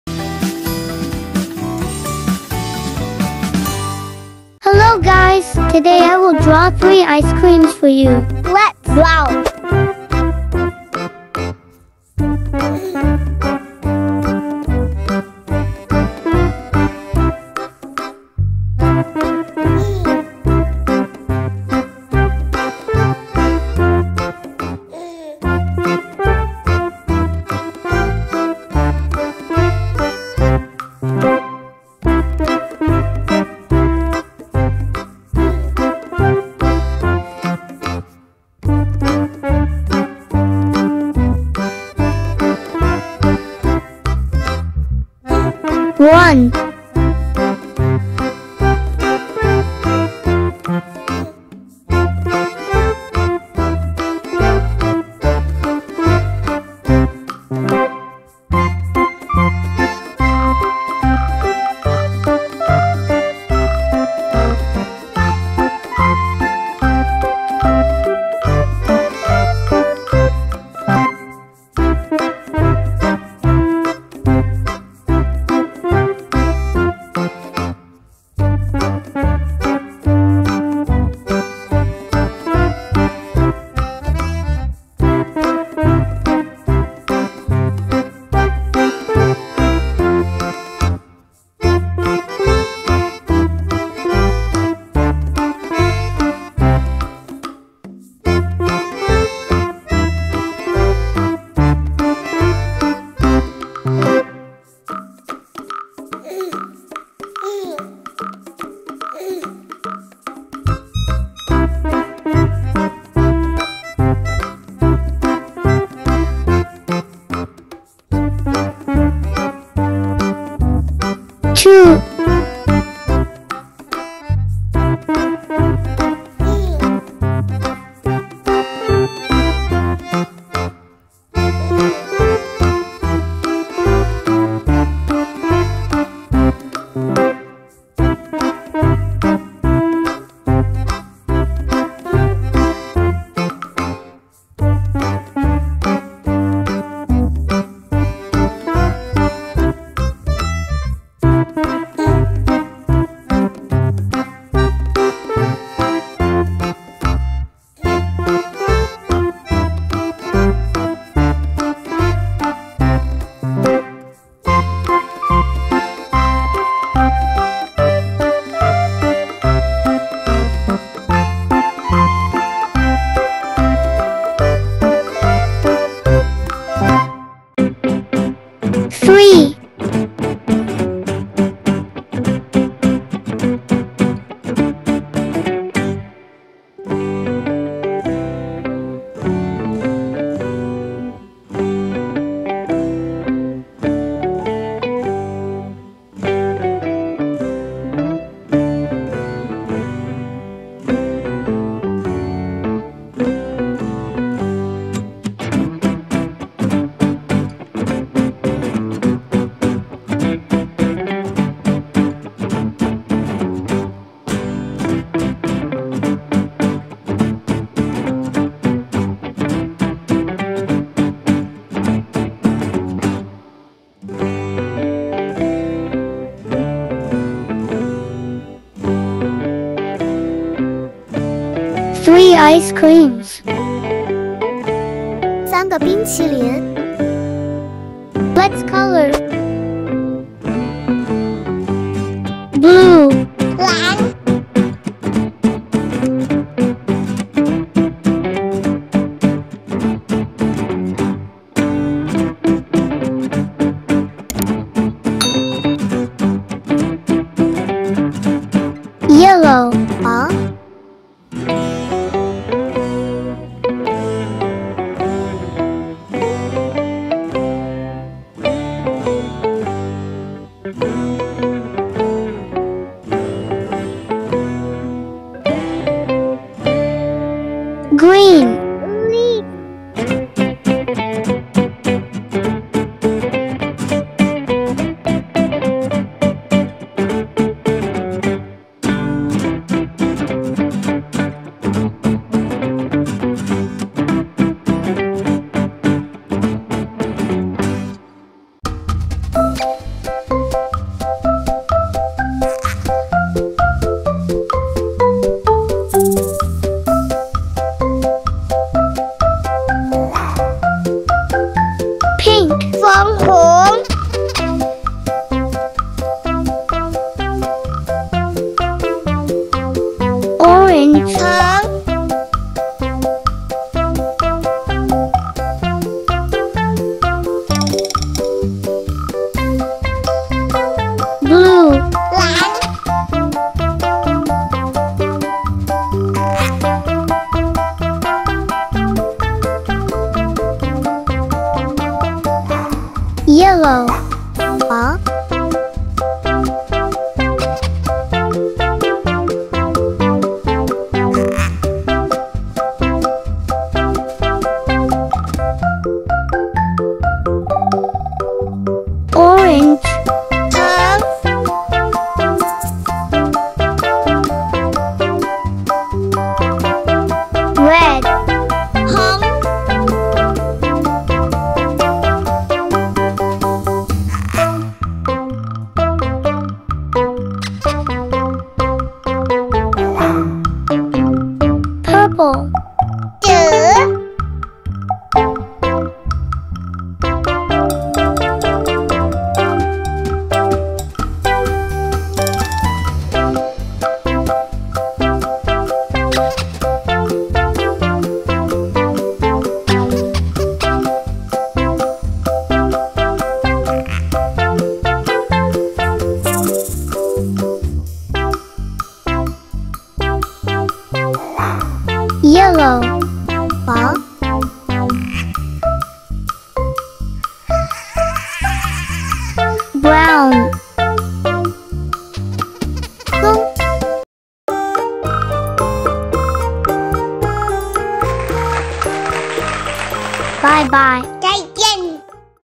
MENGGAMBAR DAN MEWARNAI 3 ESKRIM UNTUK ANAK ANAK || ICE CREAMS FOR KIDD - BECO ART
Hello guys, hari ini saya menggambar dan mewarnai eskrim. Apakah kamu suka eskrim? 😊